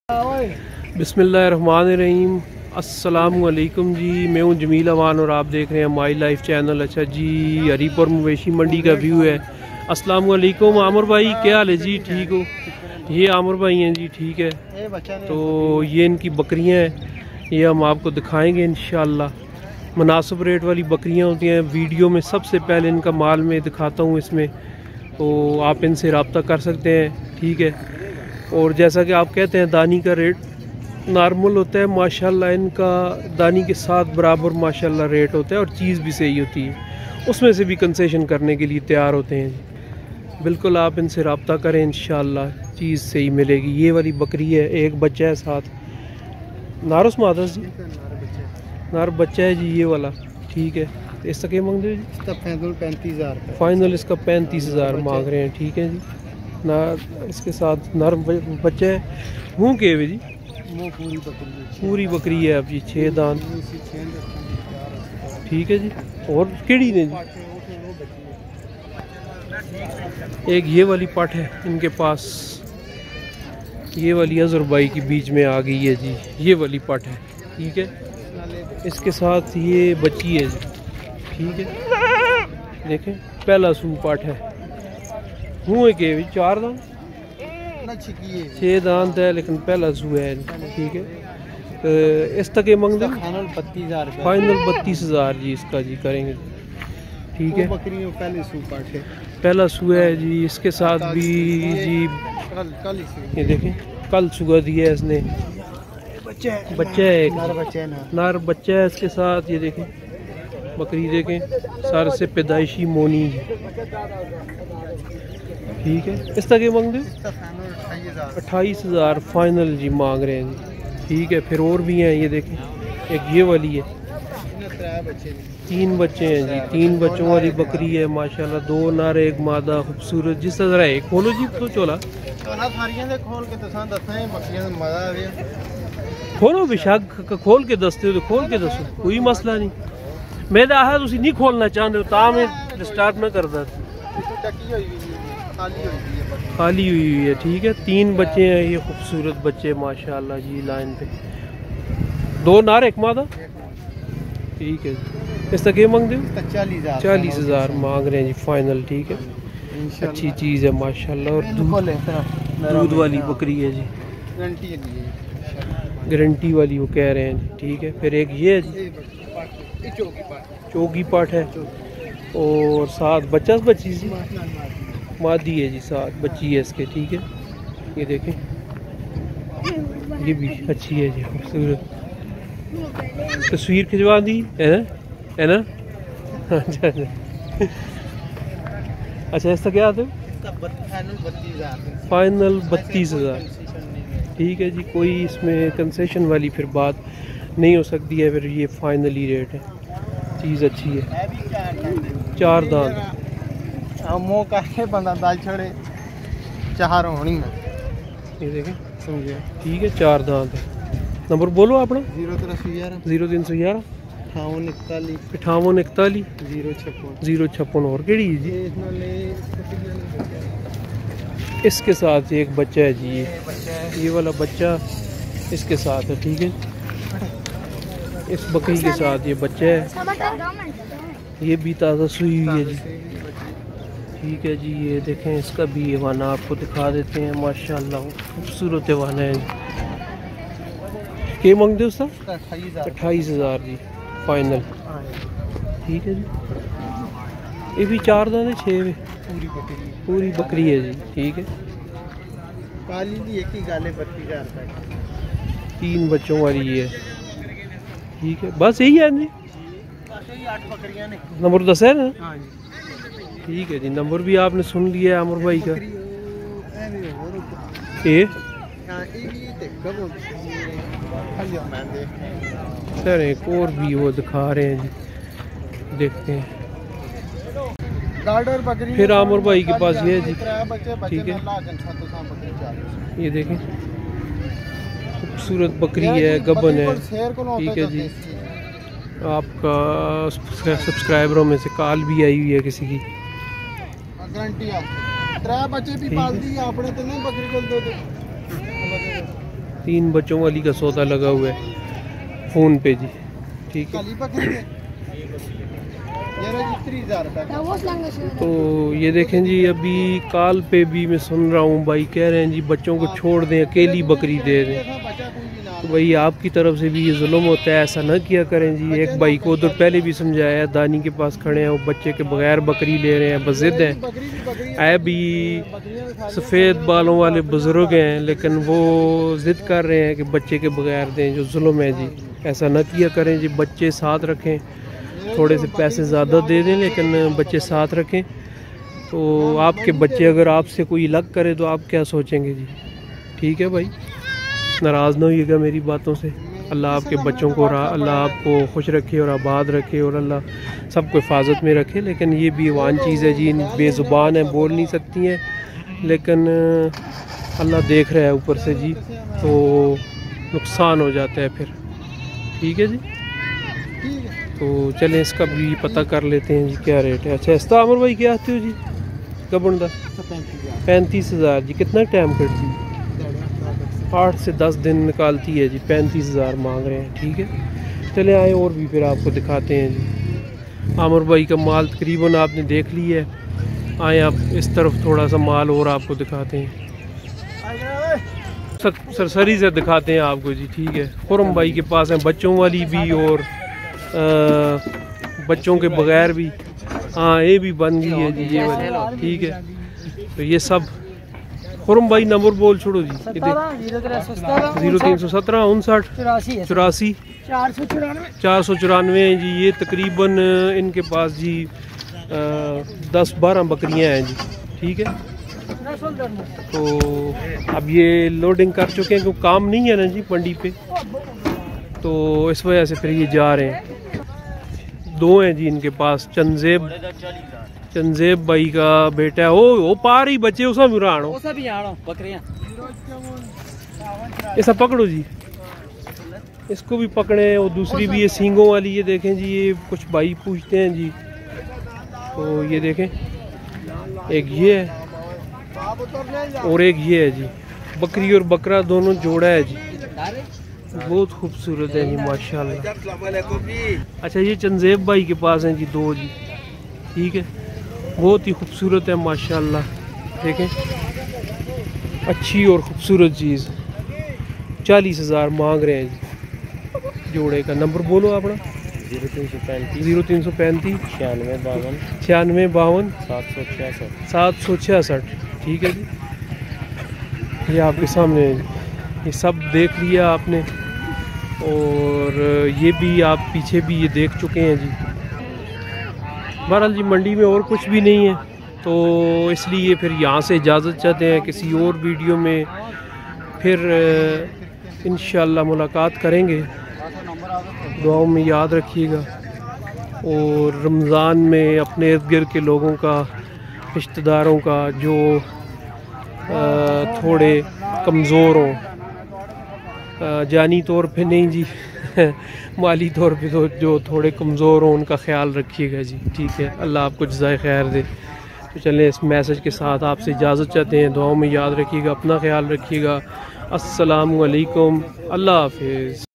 Low Price Milky And Pregnant Goats On Haripur Mandi عامر کی مناسب ریٹ بکریاں By My Life Channel
About this video:
            Beautiful milky and pregnant low price goats for sale on Haripur bakra Mandi Hazara Pakistan.